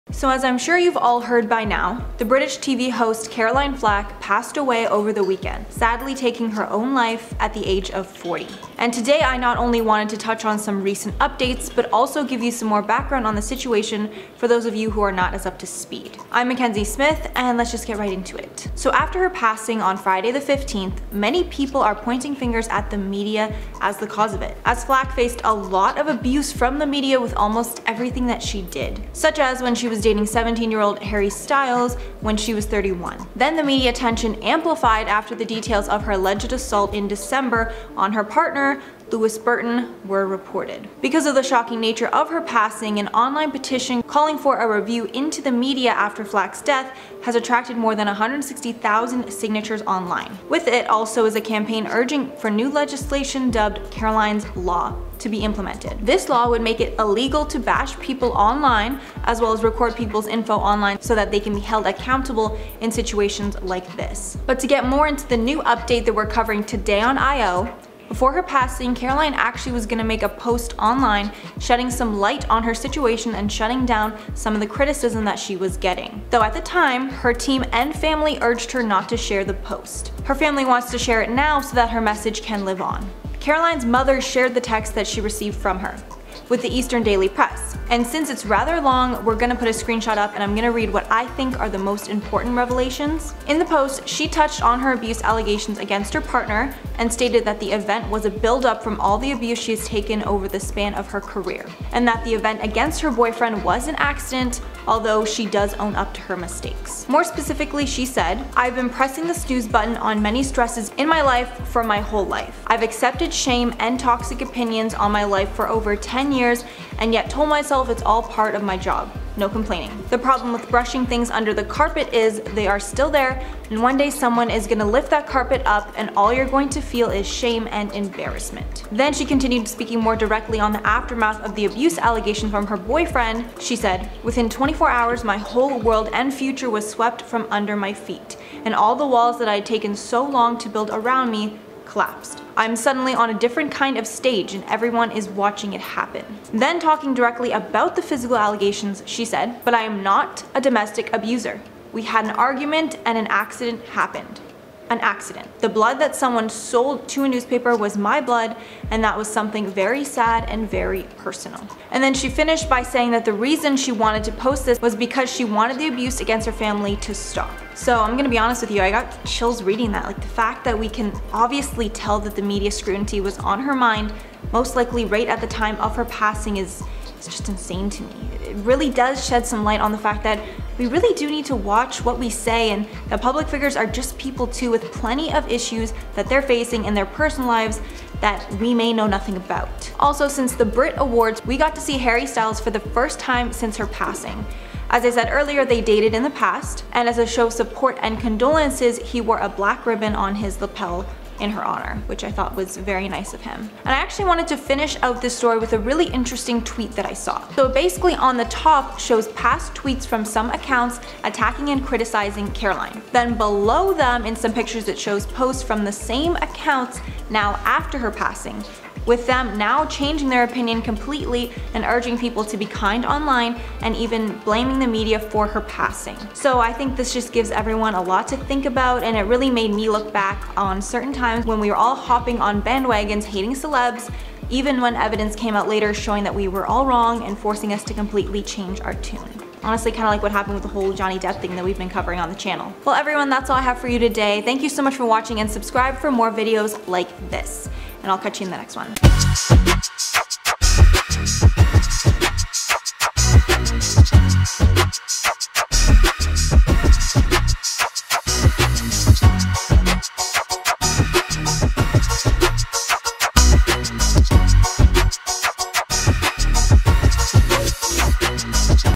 Family Of Caroline Flack Release Haunting Unpublished Instagram Post
The family of Caroline Flack have released a haunting last instagram post that was supposed to be published before she passed away.

IN THIS VIDEO:
 FILMED BY:
Chris Stiuso